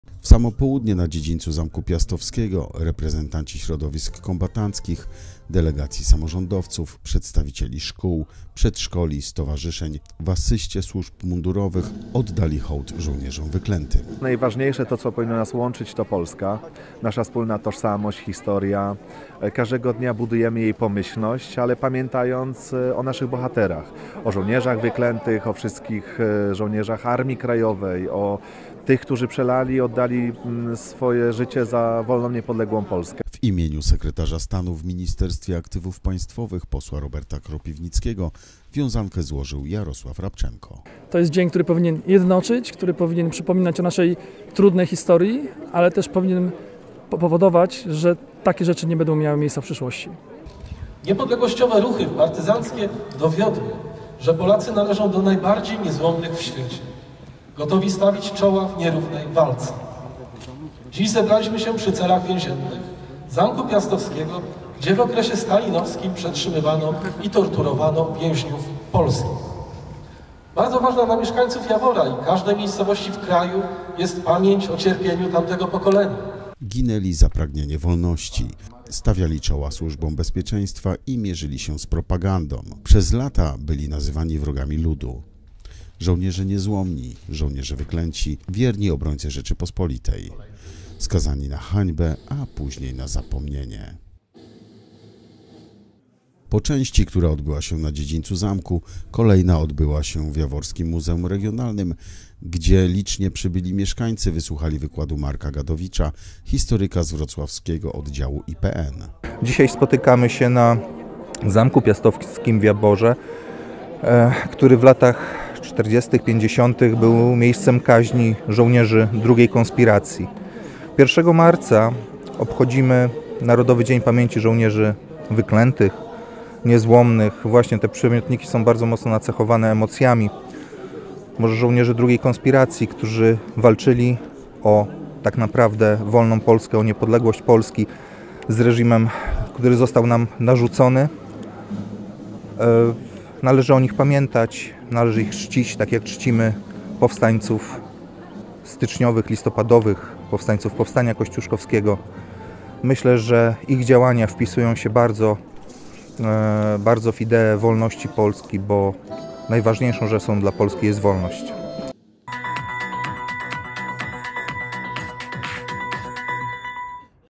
Uczcili pamięć niezłomnych
W samo południe, na dziedzińcu Zamku Piastowskiego, reprezentanci środowisk kombatanckich, delegacji samorządowców, przedstawicieliszkół, przedszkoli, stowarzyszeń, w asyście służb mundurowych jaworzanie oddali hołd Żołnierzom Wyklętym.
Po części, która odbyła się na dziedzińcu zamku, kolej odbyła się j w Muzeum Regionalnym gdzie liźnie przybyli mieszkańcy wysłuchali wykładu Marka Gadowicza, historyka z wrocławskiego oddziału IPN.